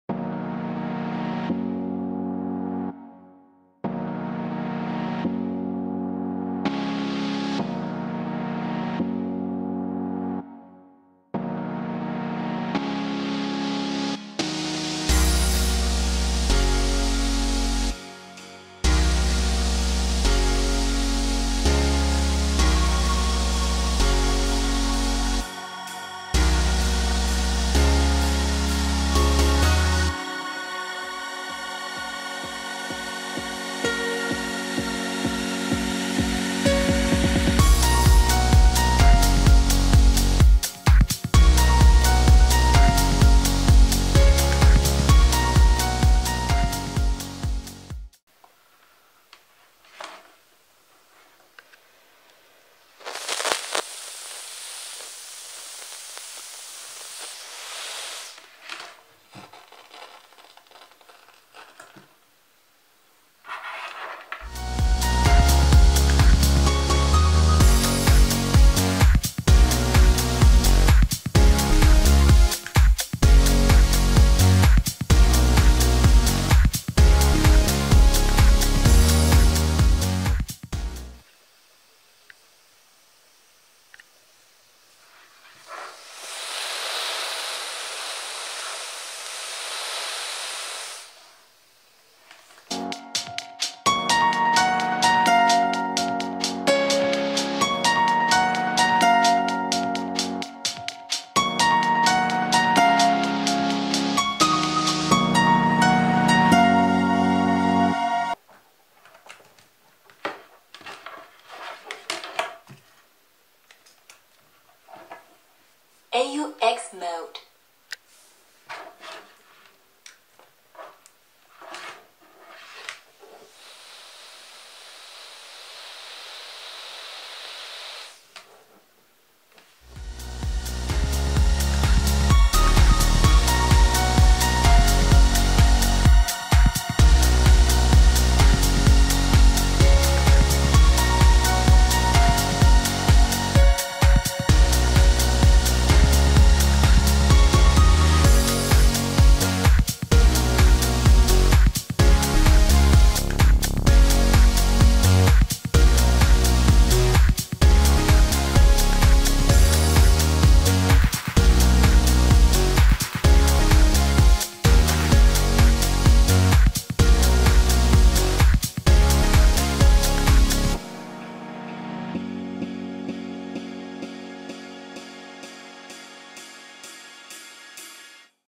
페라이트 코어 스피커 노이즈 제거의 효과 있을까?
페라이트 코어가 스피커 노이즈 제거에 효과가 있는지 테스트해 봤습니다.